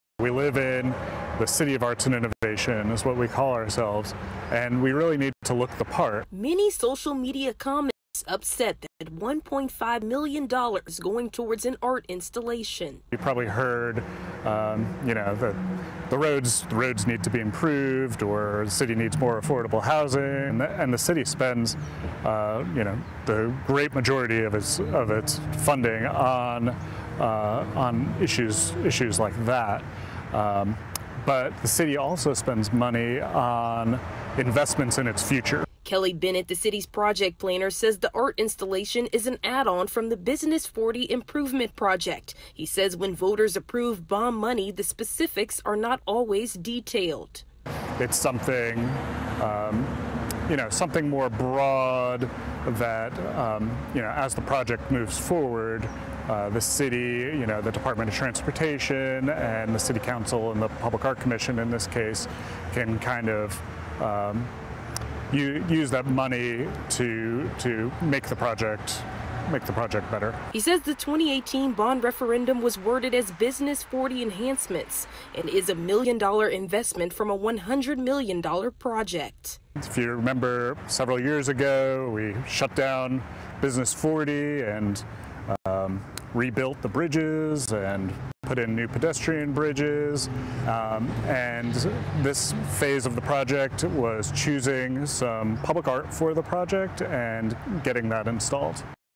Winston-Salem dandelion art stirs drama on social media
Winston-Salem's new dandelion statues are getting a lot of heat on social media, with many saying the money could be put to better use.

On Tuesday, Winston-Salem city council approved a contract to build two art installations off Salem Parkway - a 40-foot tall dandelion statue near the Peters Creek Parkway interchange and a bouquet of flowers on the Church Street bridge. The price tag - $1.05 million.

But the money for the project actually came from a bond approved by voters in 2018.

The city's project planner said while he's aware of the public's concern, the artwork is a way to grab people's attention in hopes to make the city stand out.

"We live in the city of arts and innovation, is what we call ourselves, and we really need to look the part," Kelly Bennett, with Winston-Salem Planning and Development, said.

Bennett said the art installation is an add-on from the Business 40 improvement project. He explained when voters approve bond money, the specifics are not always detailed.

"It's something more broad that as a project moves forward, the city, the department of transportation, city council, and public art commission in this case, will kind of use that money to make the project better," Bennett explained.

He said the 2018 bond referendum was worded as "Business 40 enhancements" and is a $1 million investment from a $100 million project.